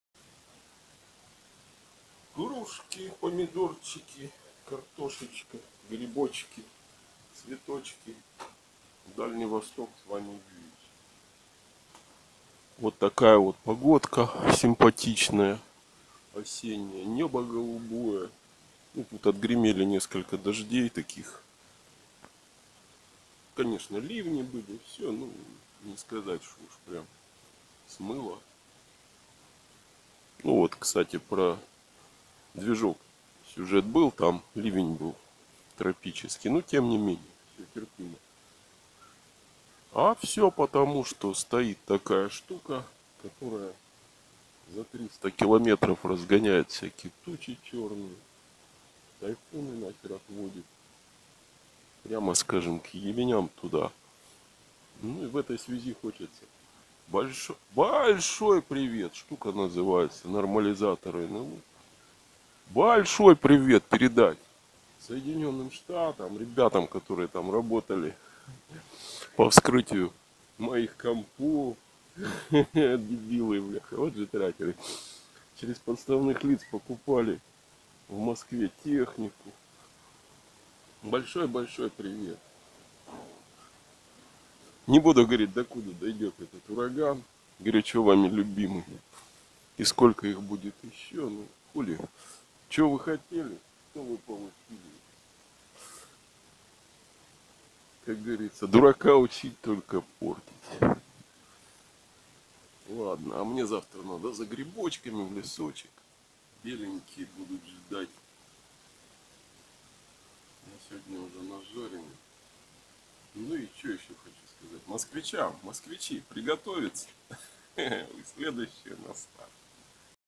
Ураганы на Флориде и нормализаторы НЛО
Стёб или не стёб - вот в чём вопрос?!
Из "потусторонних источников" "Ирма - чистка территории Атлантиды"
"Ураганы, вспышки на солнце и роль планетарных куполов"